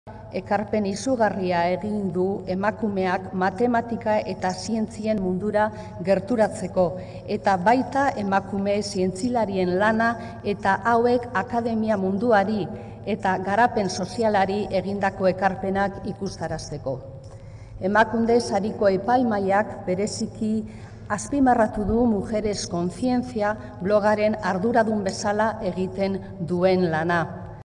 Marta Macho Stadler-en merituak Emakunderen Berdintasun Saria jasotzeko
Emakundeko zuzendaria den Izaskun Landaida Larizgoitiaren agerraldia 2016ko Berdintasunerako Emakunde sarien banaketan.

- Berdintasuna komunikabideetan.